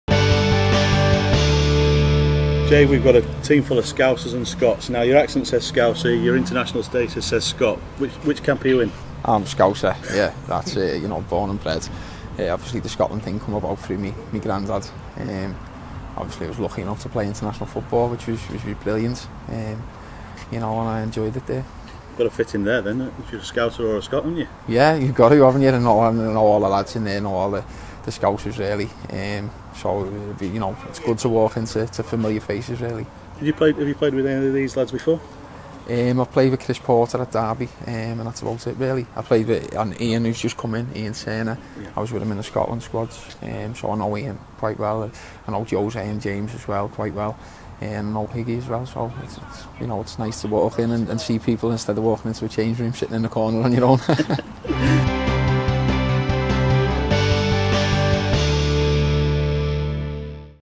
Jay McEveley - Scouse or Scot?
With the Blades dressing room falling into Scouse and Scottish numbers, new signing Jay McEveley nails his colours to the mast